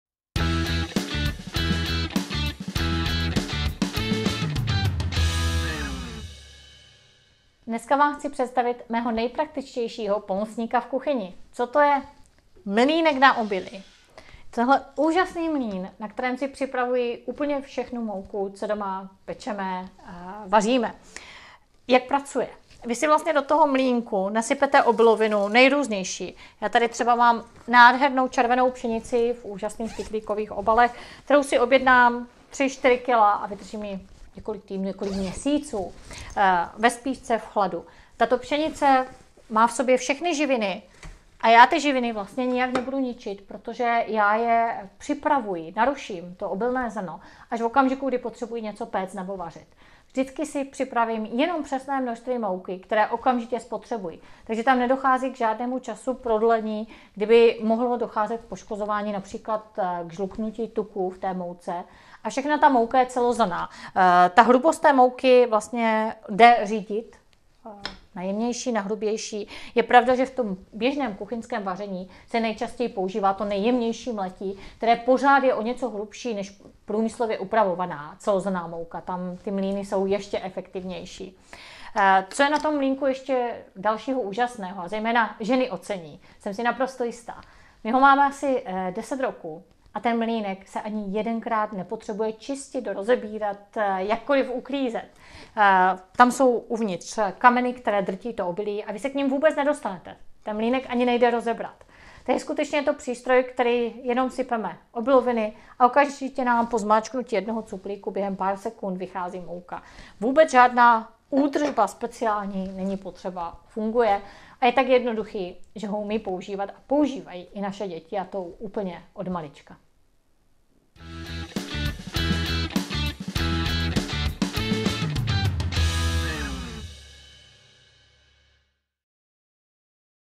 Můj nejpraktičtější pomocník v kuchyni
Mlýnek na obilí je nenahraditelným přístrojem, který v naší kuchyni pomáhá už více než 10 let. Podívejte se co zvládne a jaké má přednosti. Další videa o zdraví nejen na talíři najdete na